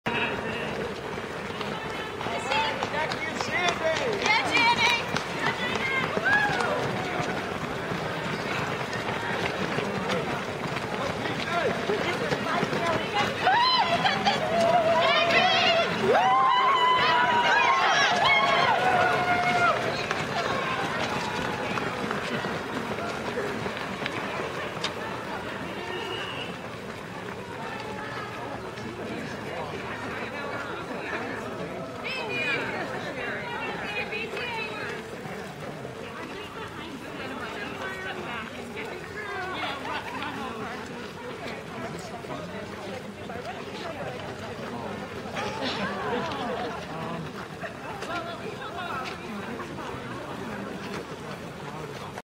Frosty Runner Road Race, Rochester, MA, Jan. 26, 2019
Runners and walkers heading to the right are in the 5K, runners and one race walker heading to the left are in the 10.5 mile race. Held at the Old Colony Regional Vocational Technical High School, Rochester, MA.